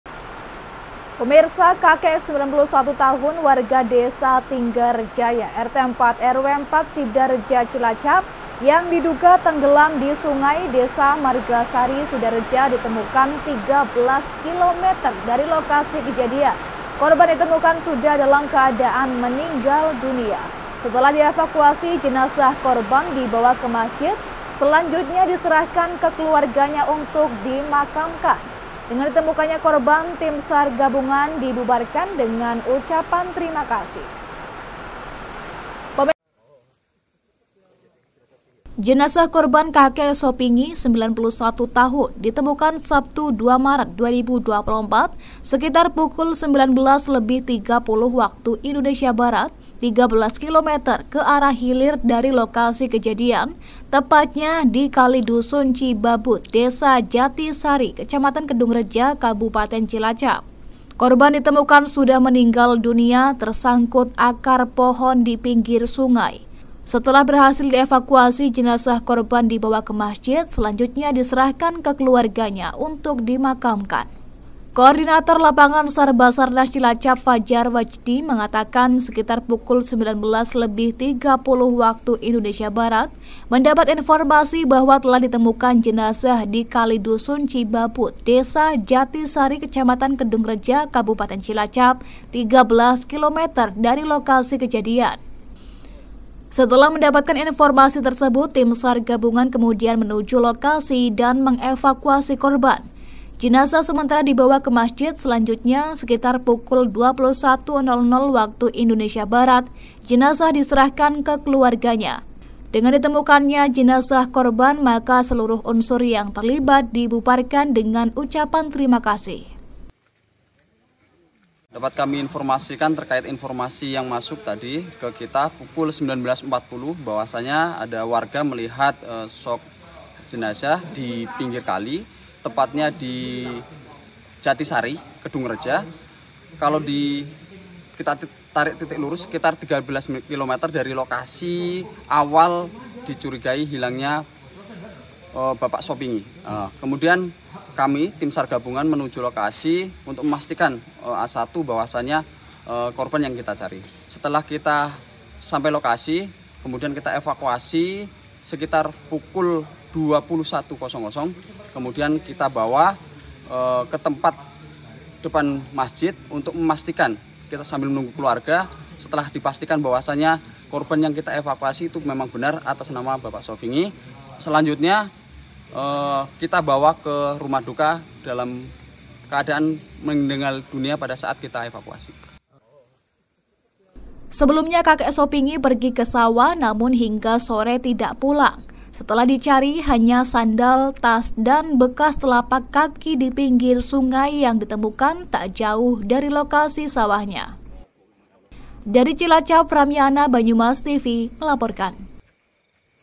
KAKEK SOPINGI, KORBAN TENGGELAM DITEMUKAN
KAKEK SOPINGI 91 TAHUN WARGA DESA TINGGARJAYA RT 04 RW 04 SIDAREJA CILACAP YANG DIDUGA TENGGELAM DI SUNGAI DESA MARGASARI SIDAREJA DITEMUKAN 13 KILO METER DARI LOKASI KEJADIAN. KORBAN DITEMUKAN SUDAH DALAM KEADAAN MENINGGAL DUNIA. SETELAH DI EVAKUASI JENAZAH KORBAN DIBAWA KE MASJID SELANJURNYA DISERAHKAN KE KELUARGANYA UNTUK DIMAKAMKAN. DENGAN DITEMUKAN KORBAN TIM SAR GABUNGAN DIBUBARKAN DENGAN UCAPAN TERIMA KASIH